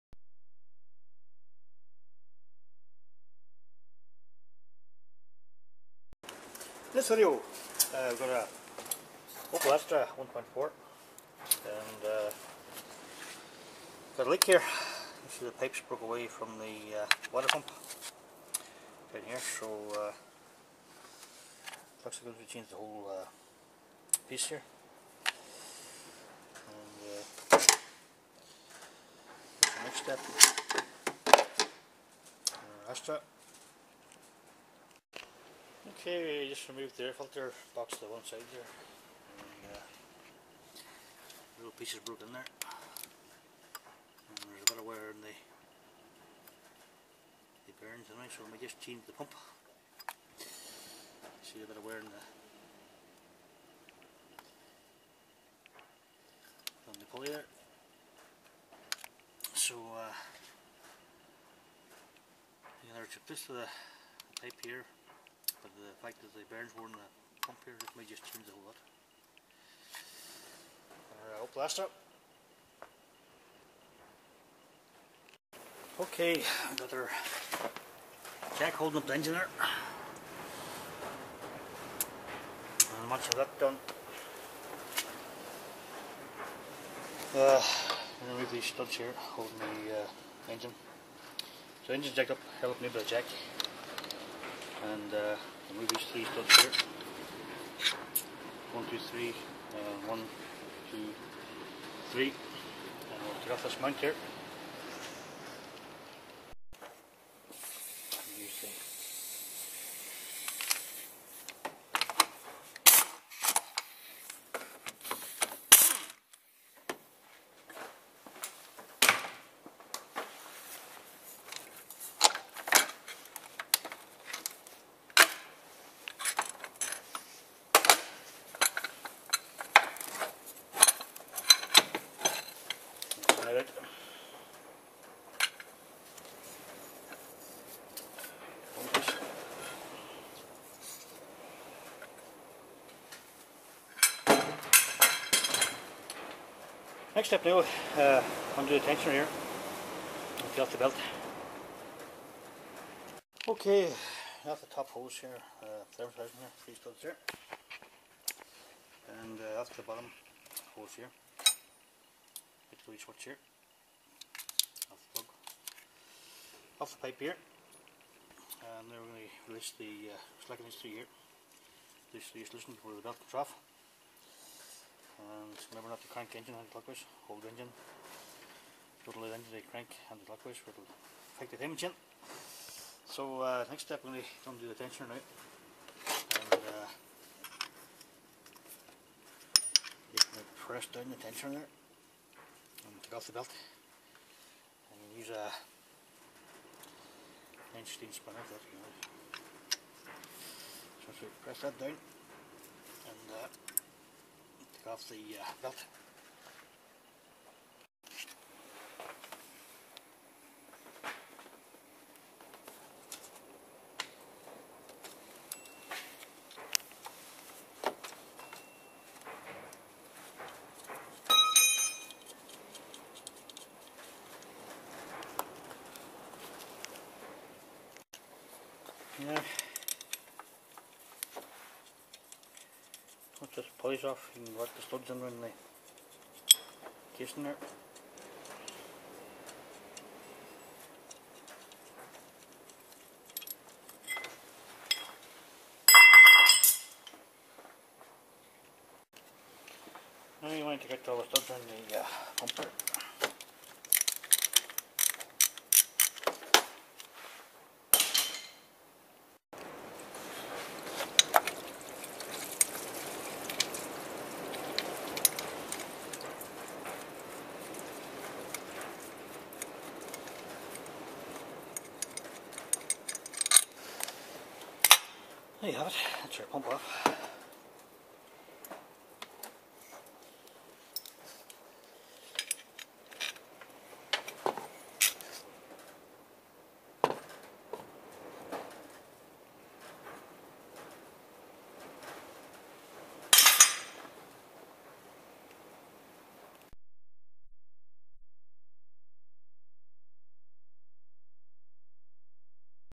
VAUXHALL ASTRA H WATER PUMP REMOVAL
vauxhall/opel astra h 1.4 water pump removal. broken bypass pipe, worn waterpump bearings 150,000 miles